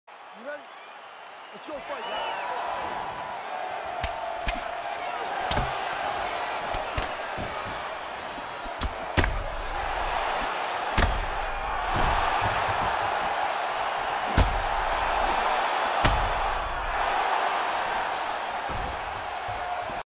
MUST SEE Brutal Knockout Of Anderson Silva By Wanderlei Silva
Anderson Silva did the Kiki Challenge while knocked out